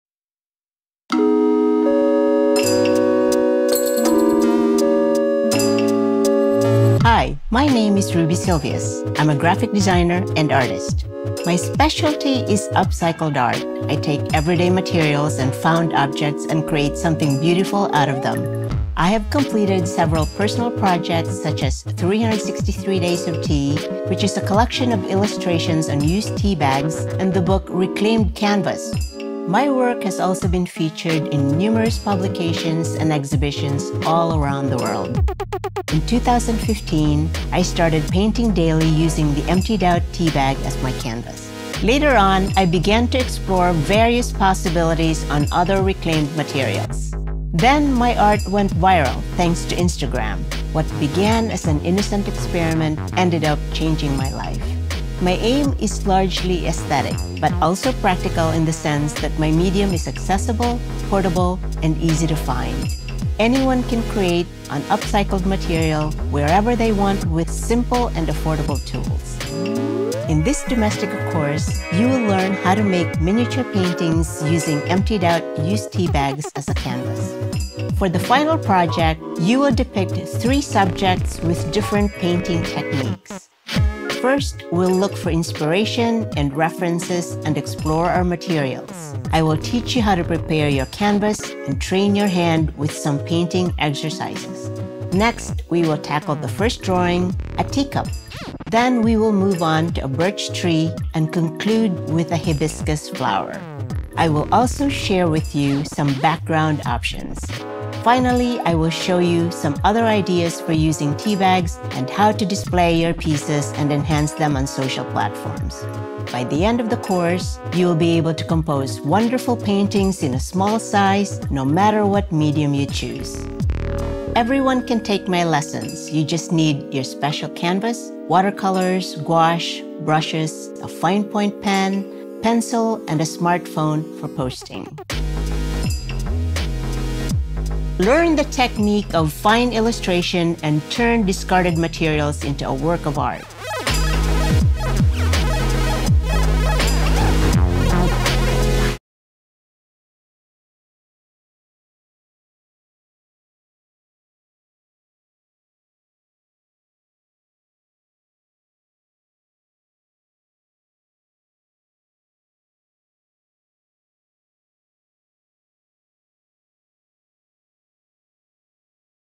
The Art of Tea Bag Painting - Course by Ruby Silvious | Domestika English
Explore watercolor and gouache to create miniature paintings using the emptied-out tea bags as a canvas

Is working with unconventional materials to create exciting and unique works of art your cup of tea? Step into Ruby Silvious’ marvelous world of upcycling. She has completed numerous projects with non-traditional materials to bring visual marvels to life, including 363 Days of Tea, a collection of illustrations with tea bags. Ruby’s inspired by everyday objects and incorporates them into her pieces. Her famous tea bag works all started when she was having a cup of tea with her sister; suddenly, the idea of using them for her art was born. Her creations have since been featured in international publications such as CNN Travel, PBS, The Guardian, Cosmopolitan China, and Vogue Italia.

In this online course, make art out of tea! Ruby takes you through different techniques for painting using tea bags as a special canvas. She teaches you how to train your hand through painting exercises and guides you through other ways you can use tea bags to create unique works of art. Once you are all done, she shows you how to display your pieces to share them with the world.